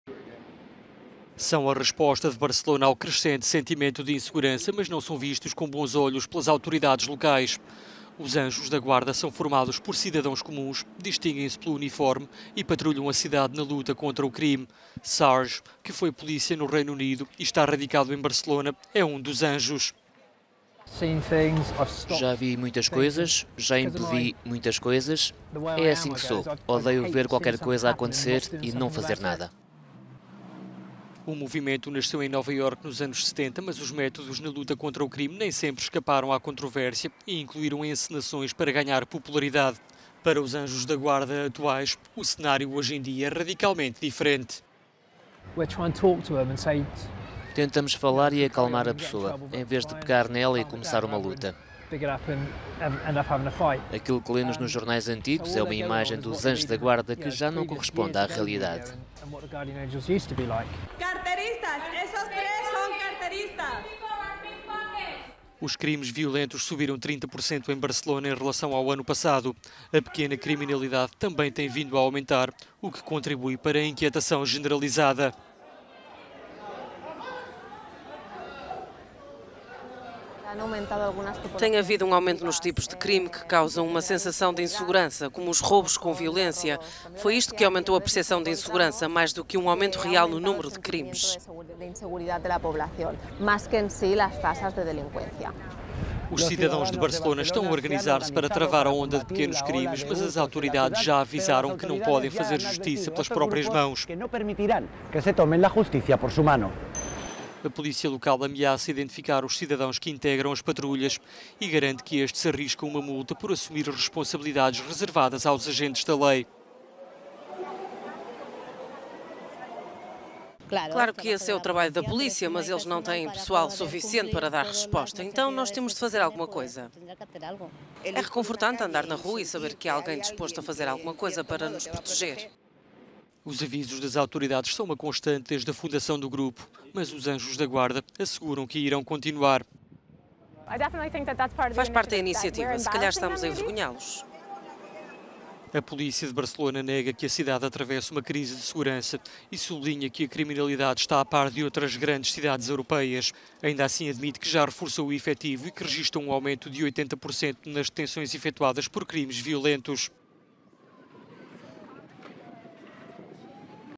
Os Anjos da Guarda de Barcelona
Grupo composto por populares participa ativamente na luta contra o crime. As autoridades não vêem a iniciativa com bons olhos…